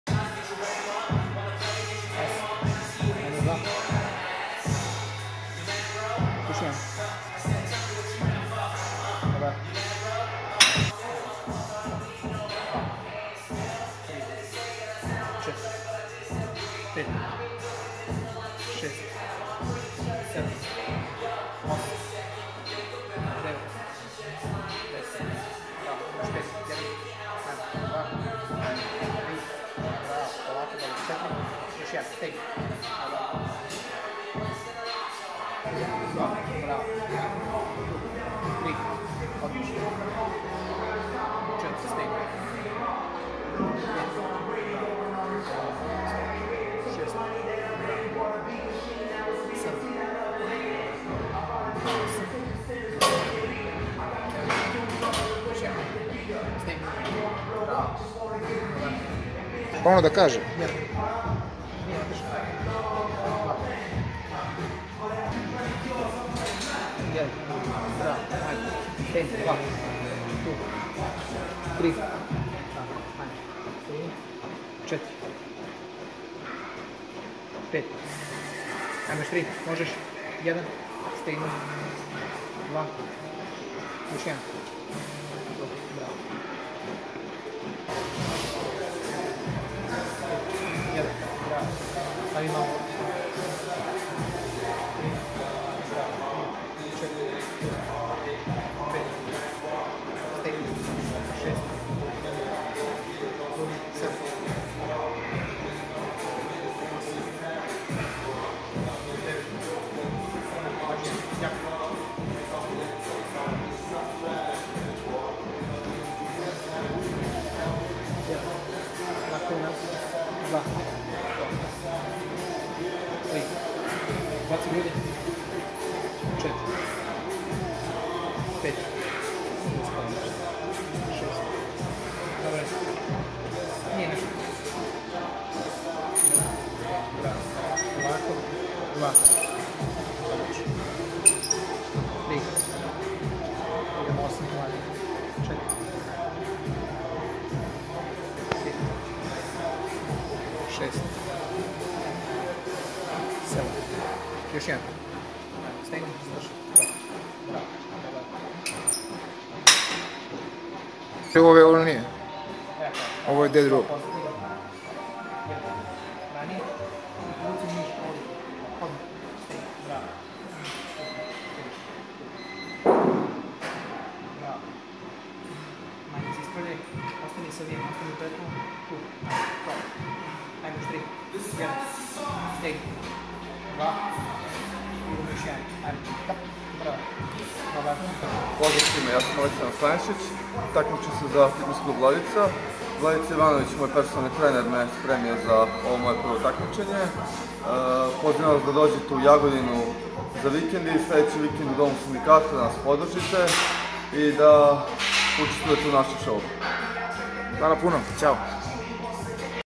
Aleksandar Stanišić bodi bilding
Mladi momak Saša Stanišić spreman je za svoje prvo takmičenje u životu, a spremao ga je Vladica Jovanović